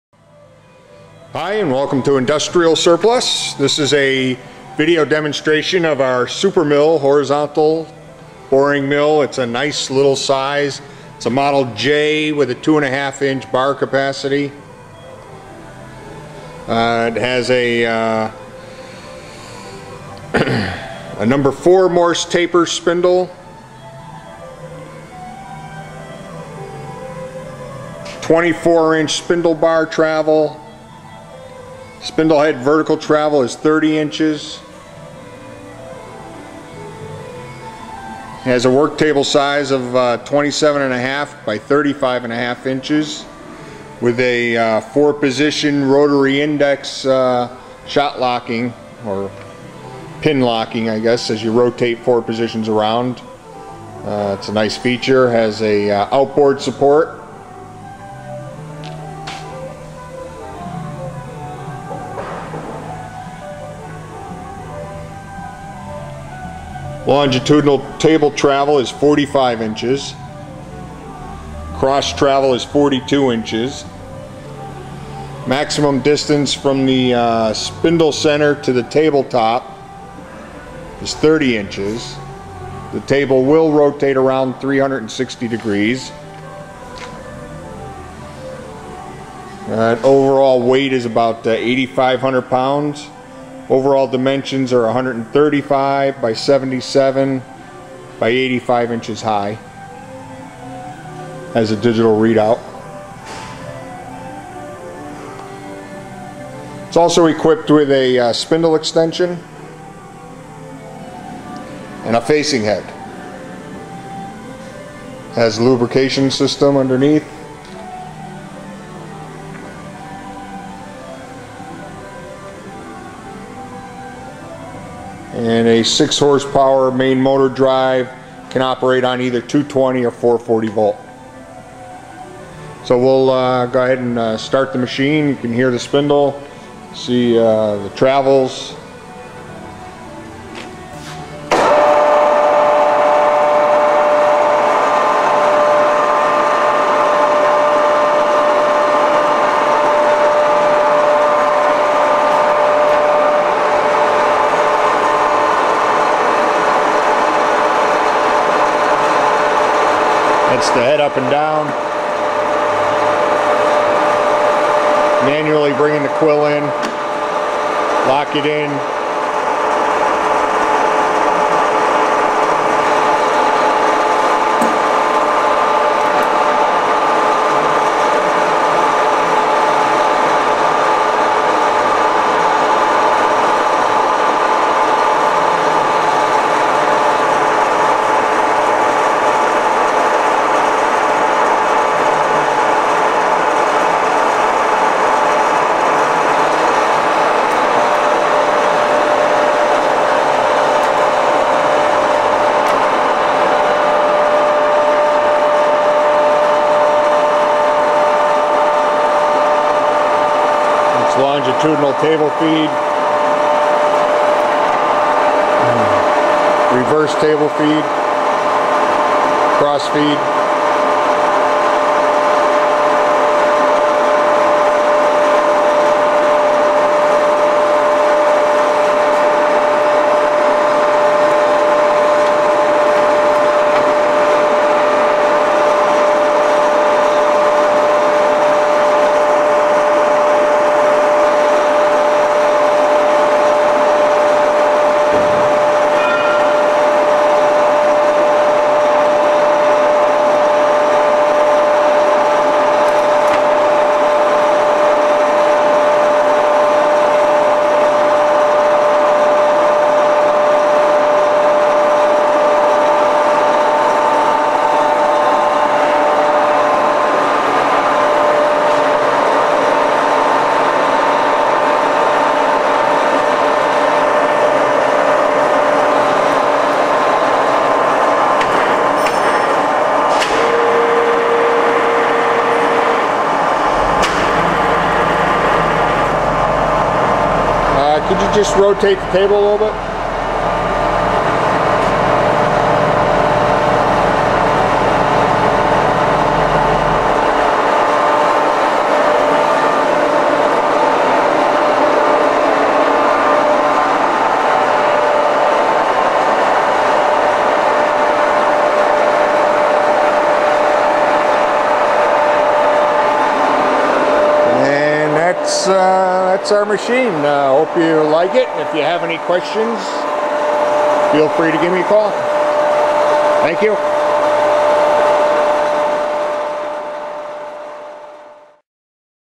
Supermill Model J, 2-1/2" Spindle, Horizontal Boring Mill, Ref.#8-33 (SOLD)
Supermill Horizontal Boring Mill    Ref.#8-33
Model J,  2-1/2"

Spindle Diameter:2-1/2"
Spindle Taper:#4 MT
Spindle Bar Travel:24"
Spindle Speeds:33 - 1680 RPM
Spindle Head Vertical Travel:30"
Power Bar Feed Rates:.0015" - .059"/ Rev.
Work Table Size:27.5" x 35.5"
Longitudinal Table Travel:45"
Cross Table Travel:42"
Max. Distance Spindle Center to Table Top:30"
Work Table Rotates:360 Degrees
Number of Shot Pin Locations for Rotary Table:4
Overall Machine Dimensions:135" x 77" x 85" High
Machine Weight, Approx.:8500 Lbs.

Equipped With:
Anilam MiniWizard 2-Axis DRO
Tailstock with Vert. Adjustment
Facing Head
Bijur Lube

6 HP Main Motor Drive    220/440/3/60